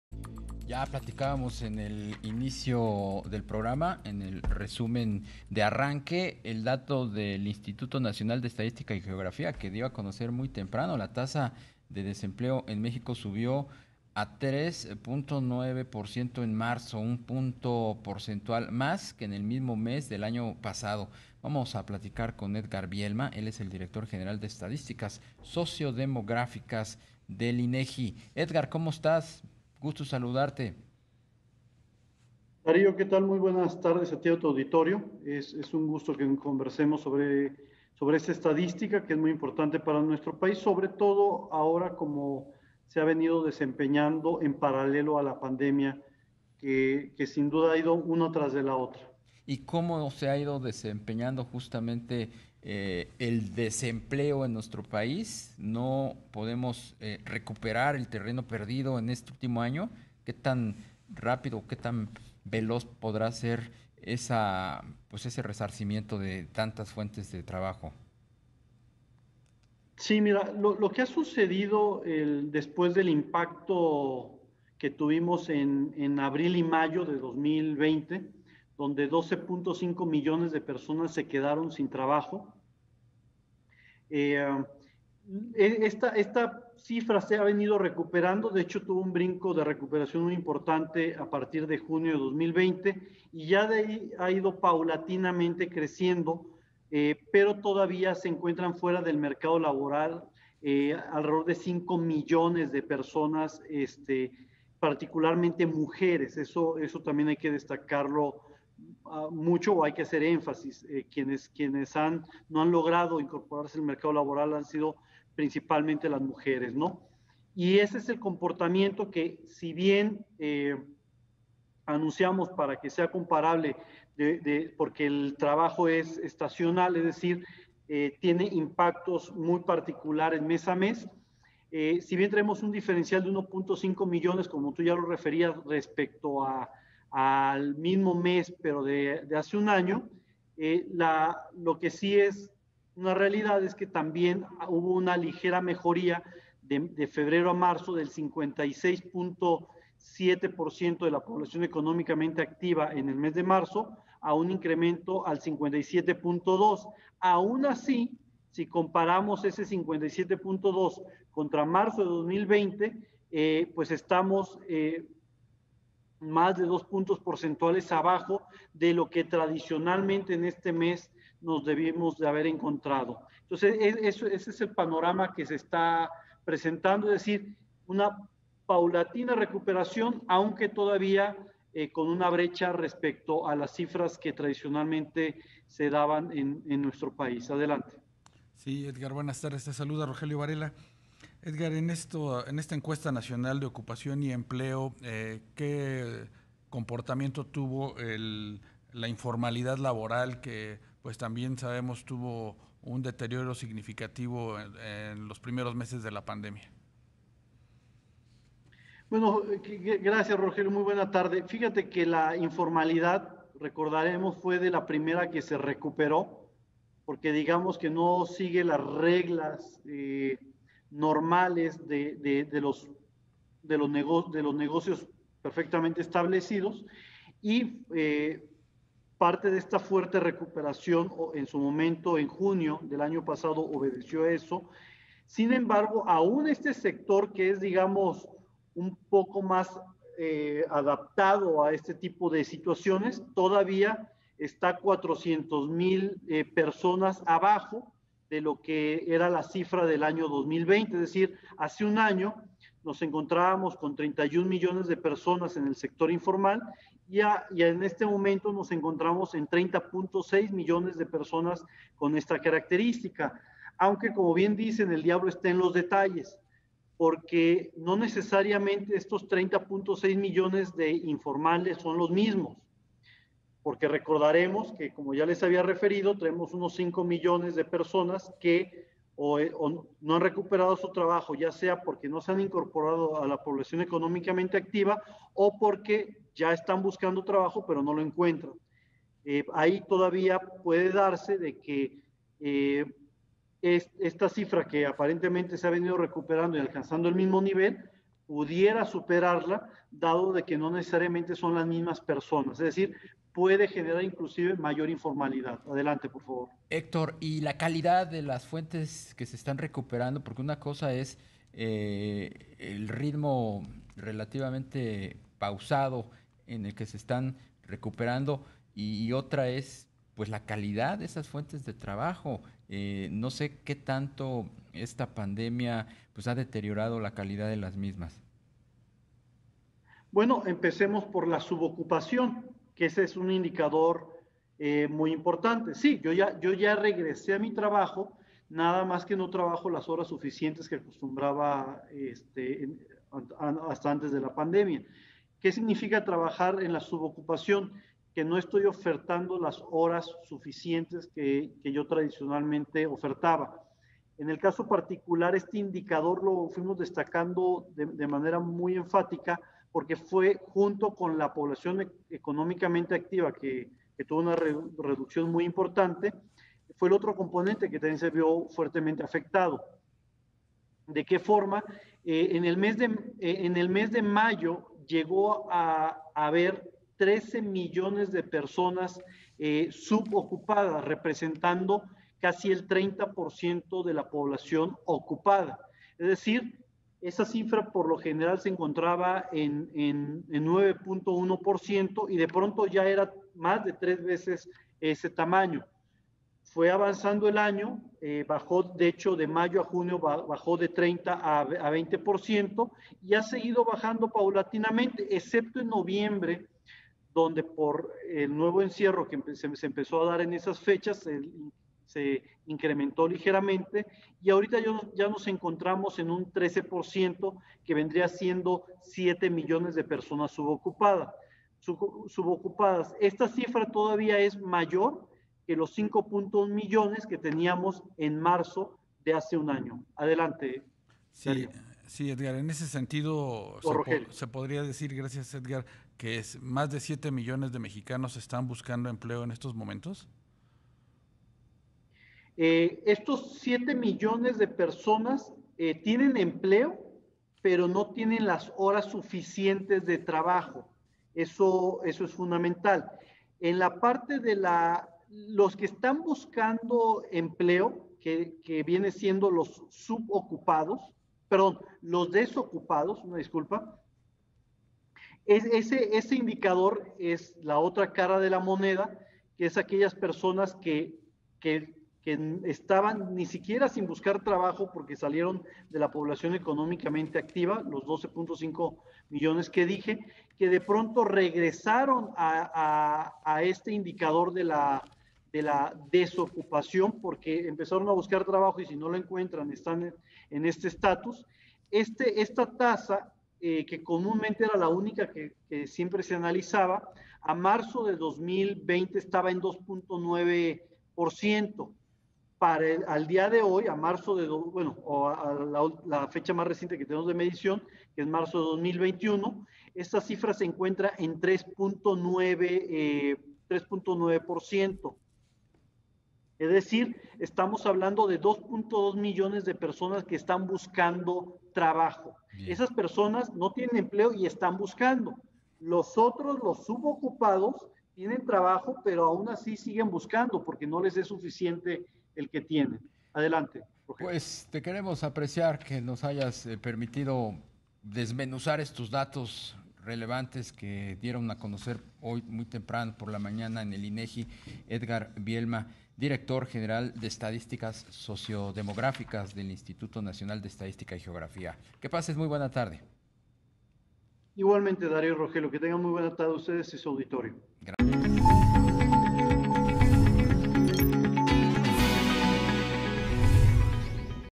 Desempeño del desempleo en México y su recuperación
Edgar Vielma, Dir. Gral. de Estadísticas Sociodemográficas del Inegi, habla en entrevista sobre el Desempeño del desempleo en México y su recuperación.

22 de abril 2021